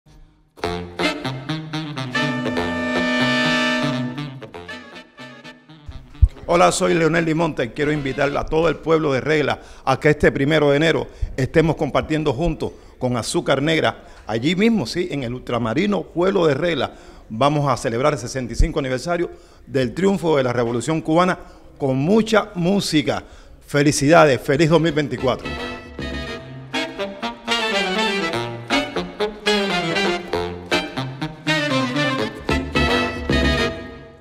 Leonel Limonta (Director de Azúcar Negra)
Músicos cubanos envían una invitación al pueblo de la capital a asistir a los espectáculos artísticos por el 1ro de Enero en saludo al Triunfo de la Revolución en diferentes municipios de La Habana